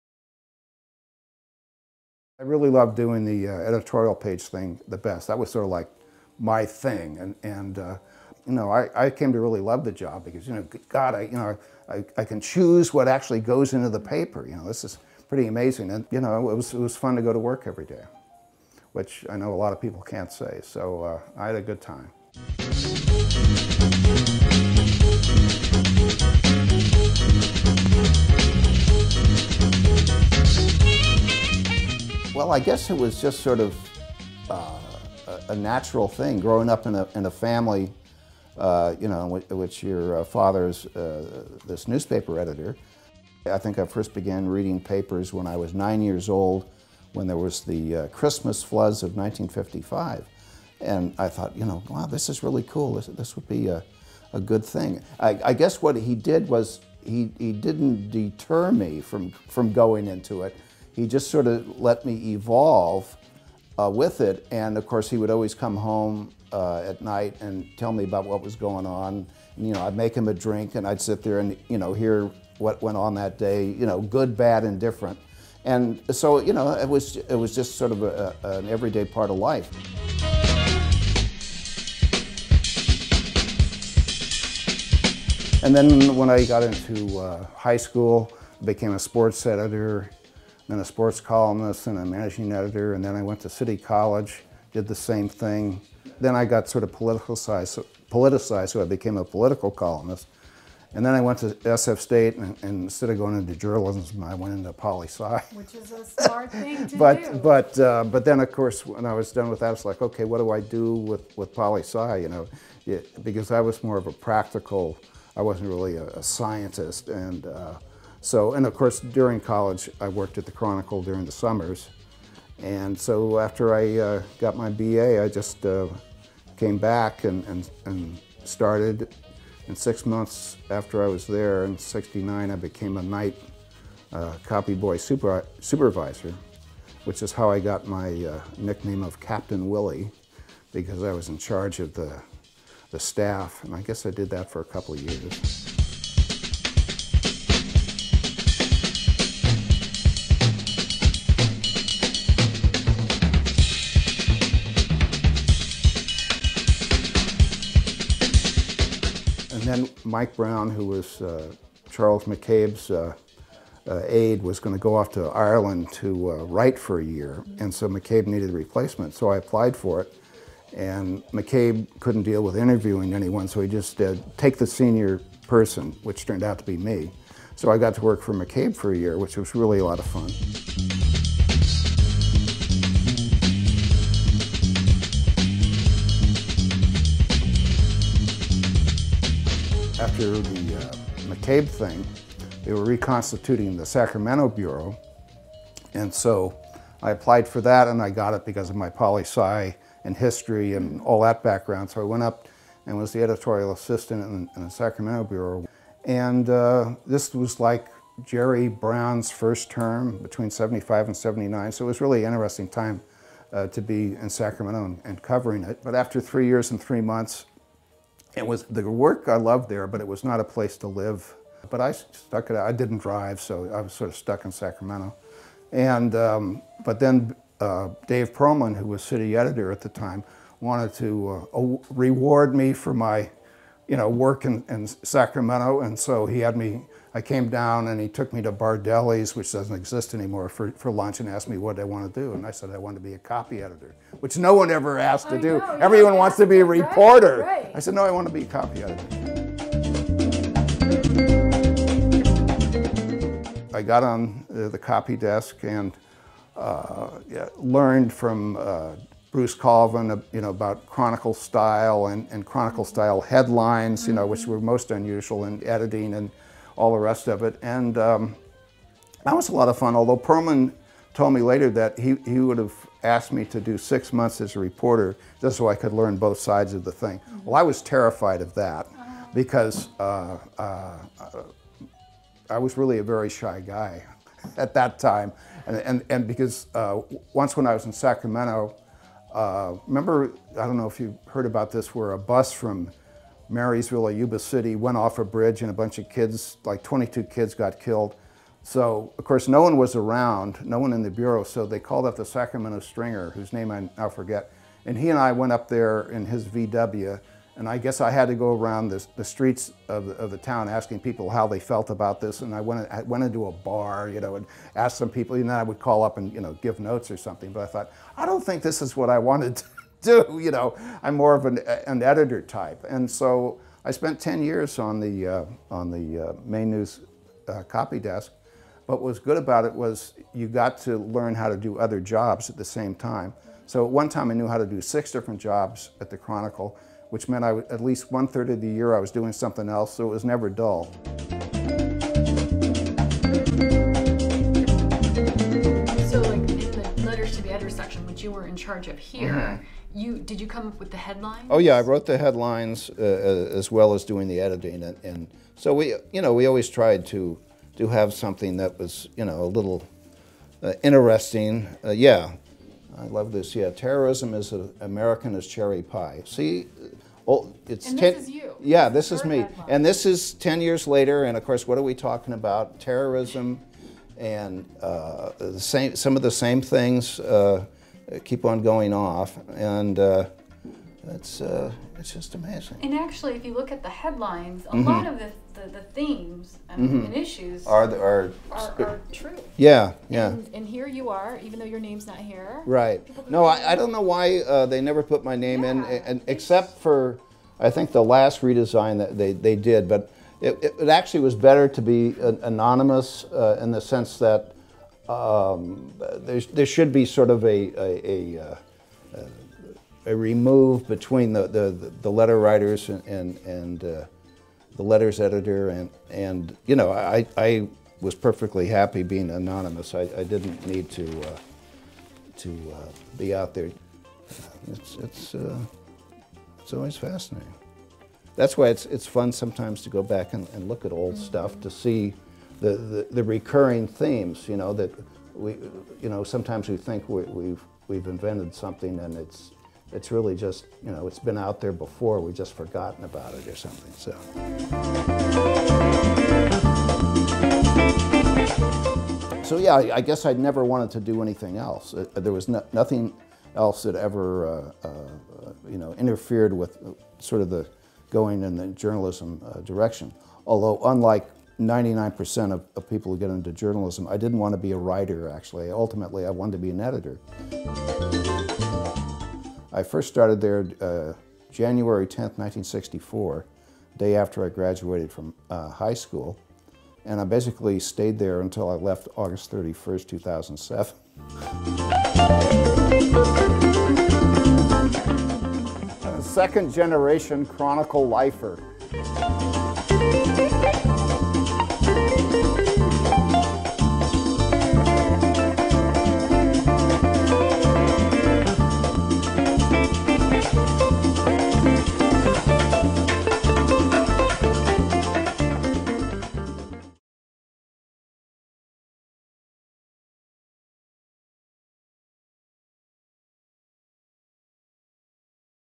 Chronicle's former letters editor Bill Pates
Bill Pates reflects on his time and dedication to journalism.